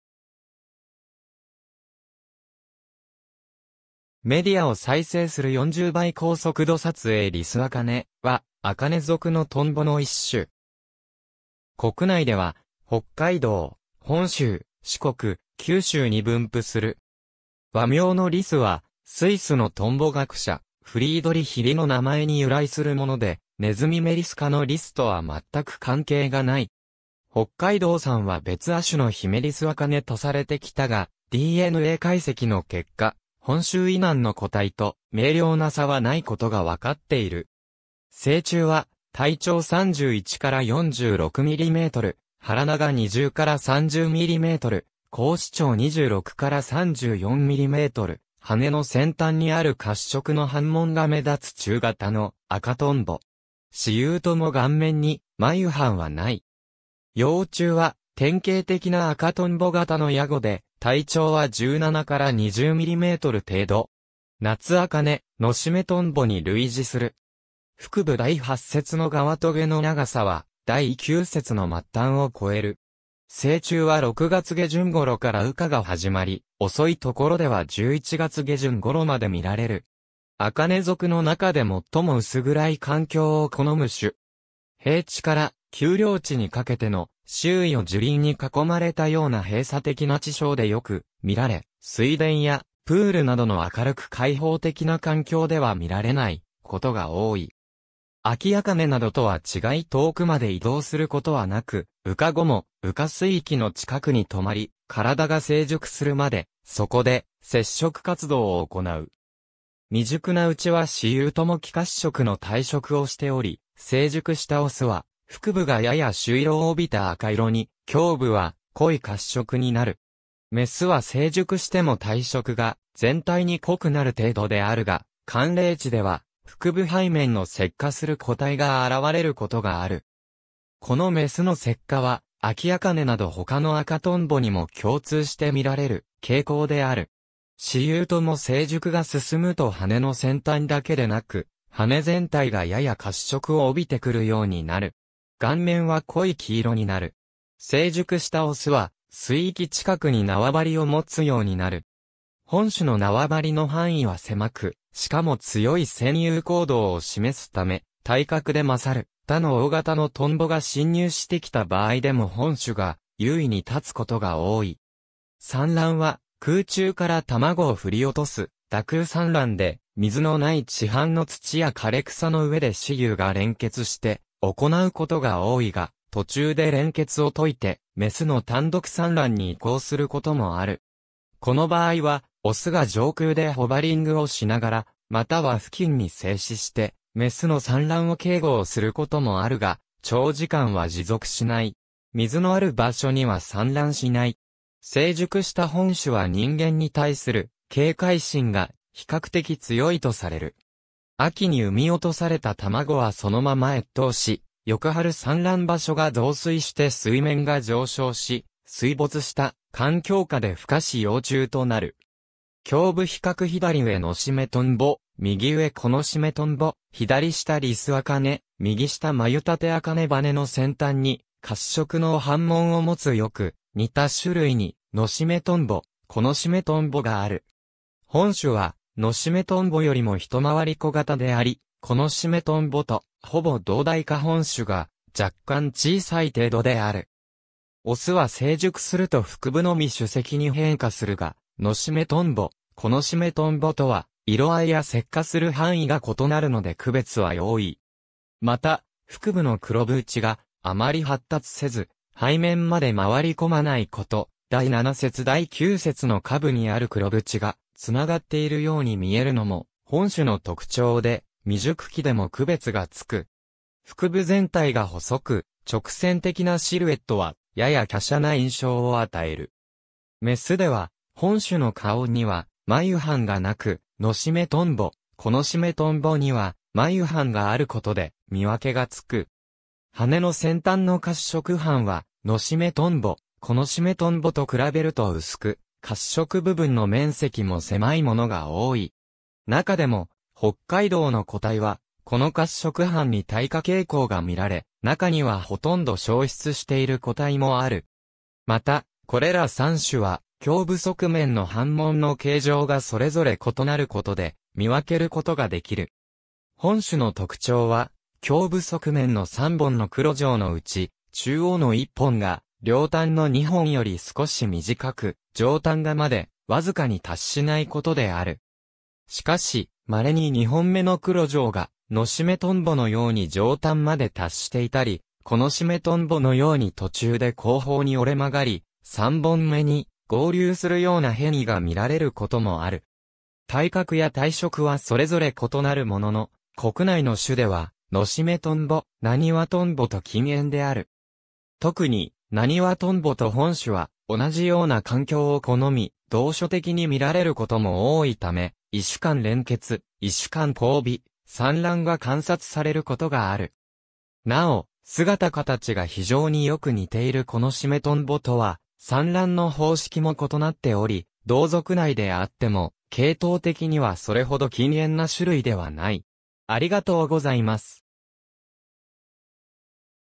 リスアカネ
メディアを再生する 40倍高速度撮影 リスアカネ（りす茜、学名 Sympetrum risi risi ）は アカネ属のトンボの一種。
国内では北海道、本州、四国、九州に分布する。
和名のリスはスイスのトンボ学者Friedrich Risの名前に由来するもので、ネズミ目リス科のリス（栗鼠）とは全く関係がない。
北海道産は別亜種のヒメリスアカネ（姫りす茜、学名 Sympetrum risi yosico ）とされてきたが、DNA解析の結果、本州以南の個体と明瞭な差はないことがわかっている。
成虫は、体長31-46mm、腹長20-30mm、後翅長26-34mm、翅の先端にある褐色の斑紋が目立つ中型の赤とんぼ。
雌雄とも顔面に眉班（ビハン）はない。
幼虫は典型的な赤とんぼ型のヤゴで、体長は17-20mm程度。
ナツアカネ、ノシメトンボに類似する。
腹部第8節の側棘の長さは第9節の末端を超える。
成虫は6月下旬頃から羽化が始まり、遅いところでは11月下旬頃まで見られる。
アカネ属の中で最も薄暗い環境を好む種。
平地から丘陵地にかけての、周囲を樹林に囲まれたような閉鎖的な池沼でよく見られ、水田やプールなどの明るく開放的な環境では見られないことが多い。
アキアカネなどとは違い遠くまで移動することはなく、羽化後も羽化水域の近くに留まり、体が成熟するまでそこで摂食活動を行う。
未熟なうちは雌雄とも黄褐色の体色をしており、成熟した雄は腹部がやや朱色を帯びた赤色に、胸部は濃い褐色になる。
雌は成熟しても体色が全体に濃くなる程度であるが、寒冷地では腹部背面の赤化する個体が現れることがある。
この雌の赤化はアキアカネなど他の赤とんぼにも共通して見られる傾向である。
雌雄とも成熟が進むと翅の先端だけでなく、翅全体がやや褐色を帯びてくるようになる。
顔面は濃い黄色になる。
成熟した雄は水域近くに縄張りを持つようになる。
本種の縄張りの範囲は狭く、しかも強い占有行動を示すため、体格で勝る他の大型のトンボが侵入してきた場合でも本種が優位に立つことが多い。
産卵は空中から卵を振り落とす打空産卵で、水のない池畔の土や枯れ草の上で雌雄が連結して行うことが多いが、途中で連結を解いて雌の単独産卵に移行することもある。
この場合は雄が上空でホバリングをしながら、または付近に静止して雌の産卵を警護をすることもあるが、長時間は持続しない。
水のある場所には産卵しない。
成熟した本種は人間に対する警戒心が比較的強いとされる。
秋に産み落とされた卵はそのまま越冬し、翌春産卵場所が増水して水面が上昇し水没した環境下で孵化し幼虫となる。
胸部比較 左上)ノシメトンボ・右上)コノシメトンボ ・左下)リスアカネ・右下)マユタテアカネ 翅の先端に褐色の斑紋を持つ良く似た種類にノシメトンボ、コノシメトンボがある。
本種はノシメトンボよりもひと回り小型であり、コノシメトンボとほぼ同大か本種が若干小さい程度である。
雄は成熟すると腹部のみ朱赤に変化するが、ノシメトンボ（わずかに赤みがさす程度）、コノシメトンボ（全身が赤化し、やや黒味を帯びた赤）とは色合いや赤化する範囲が異なるので区別は容易。
また、腹部の黒斑があまり発達せず背面まで回り込まないこと、第7節～第9節の下部にある黒斑が繋がっているように見えるのも本種の特徴で、未熟期でも区別がつく。
腹部全体が細く直線的なシルエットは、やや華奢な印象を与える。
雌では、本種の顔には眉斑がなく、ノシメトンボ、コノシメトンボには眉斑があることで見分けがつく。
翅の先端の褐色斑は、ノシメトンボ、コノシメトンボと比べると薄く、褐色部分の面積も狭いものが多い。
中でも北海道の個体は、この褐色斑に退化傾向が見られ、中にはほとんど消失している個体もある。
また、これら三種は胸部側面の斑紋の形状がそれぞれ異なることで見分けることができる。
本種の特徴は、胸部側面の3本の黒条のうち、中央の1本が両端の2本より少し短く、上端までわずかに達しないことである。
しかし、まれに2本目の黒条がノシメトンボのように上端まで達していたり（雌に比較的多い）、コノシメトンボのように途中で後方に折れ曲がり、3本目に合流するような変異（雄に比較的多い）が見られることもある。
体格や体色はそれぞれ異なるものの、国内の種ではノシメトンボ、ナニワトンボと近縁である。
特にナニワトンボと本種は同じような環境を好み、同所的に見られることも多いため、異種間連結・異種間交尾・産卵が観察されることがある。
尚、姿形が非常によく似ている...